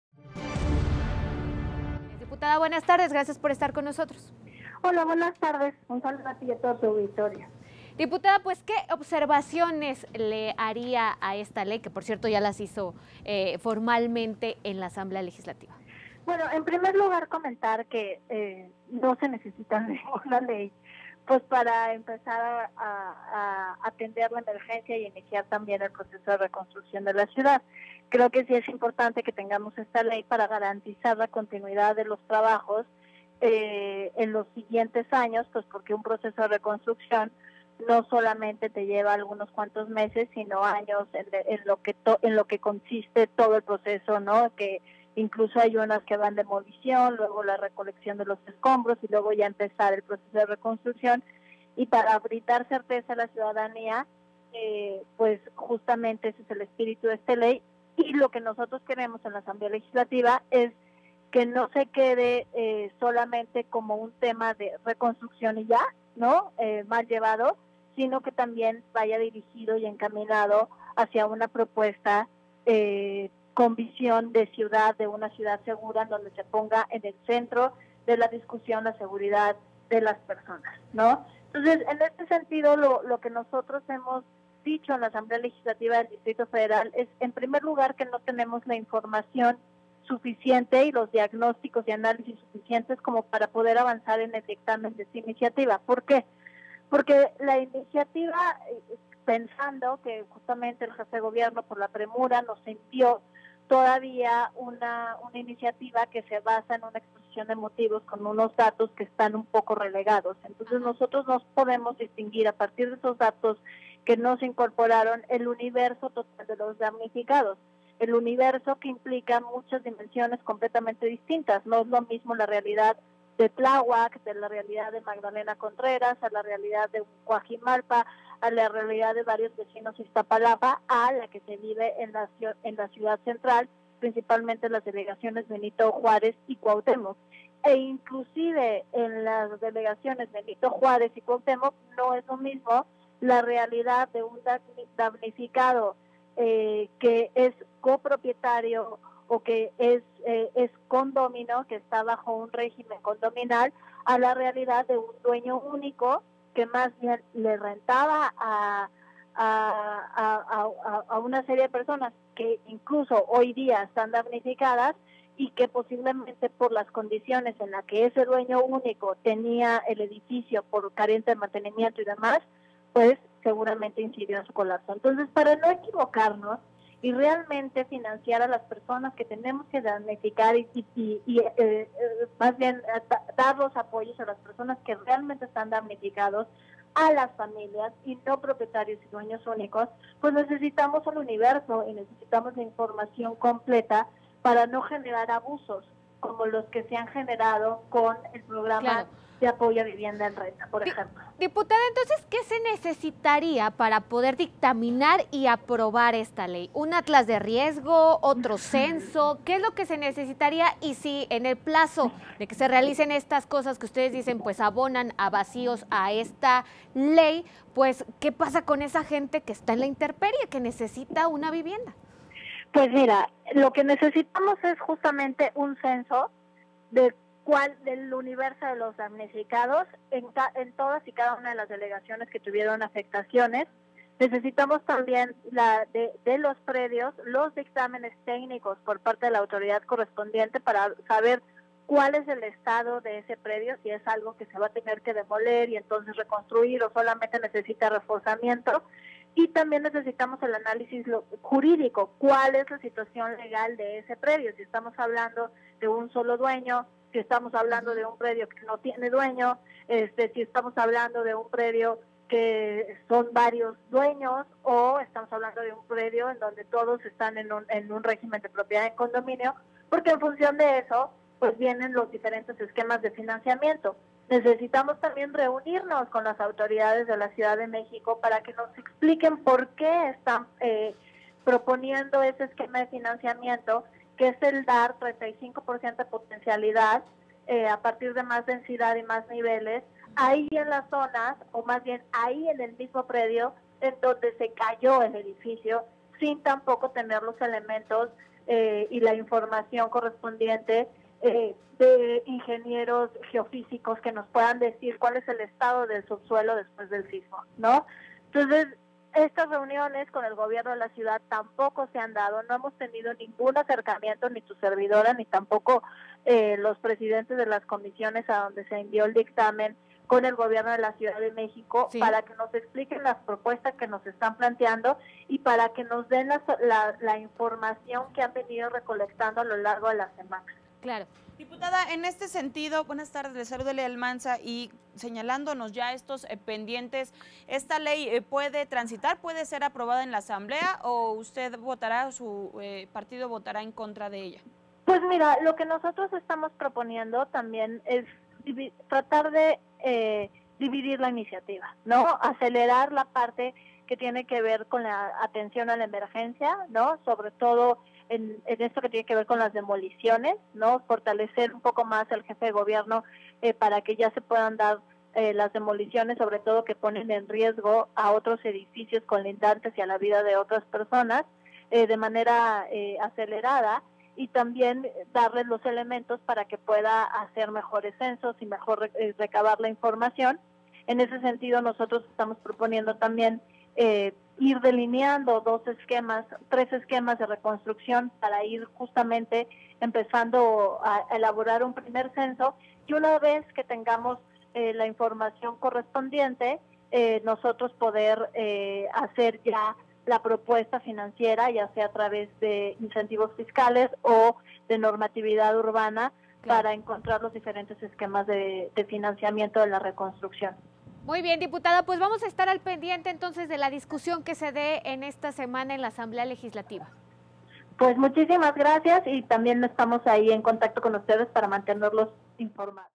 Entrevista Dunia Ludlow - Ley de Reconstrucción - 30 de octubre
Dunia Ludlow Deloya, diputada del PRI en la ALDF y Presidenta de la comisión de vivienda, nos habla sobre la Ley de Reconstrucción enviada por el ejecutivo local a la Asamblea Legislativa ¿Qué observaciones le haría a esta ley? ¿Habrá beneficios para la ciudadanía con esta legislación?